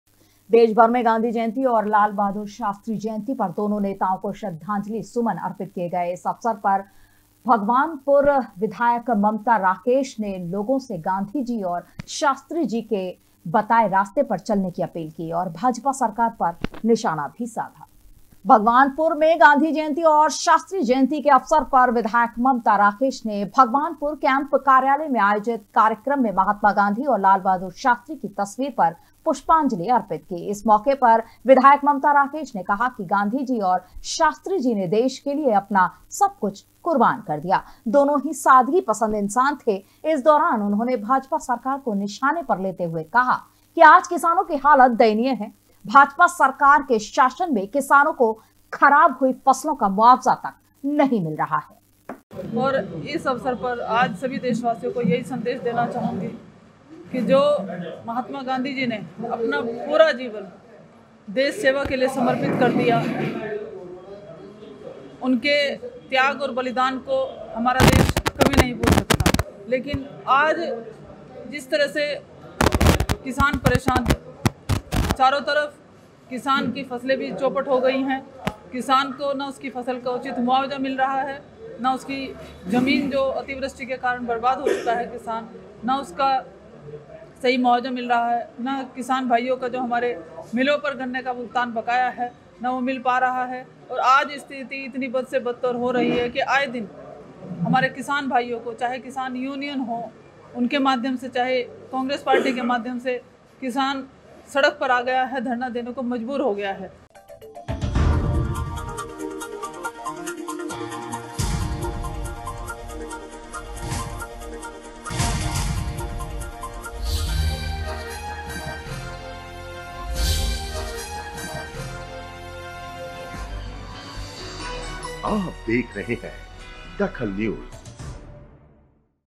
DAKHAL NEWS गाँधी जयंती और शास्त्री जयंती पर हुए कार्यक्रम
गाँधी जयंती और शास्त्री जयंती पर हुए  कार्यक्रम
विधायक ममता राकेश ने भाजपा पर साधा निशाना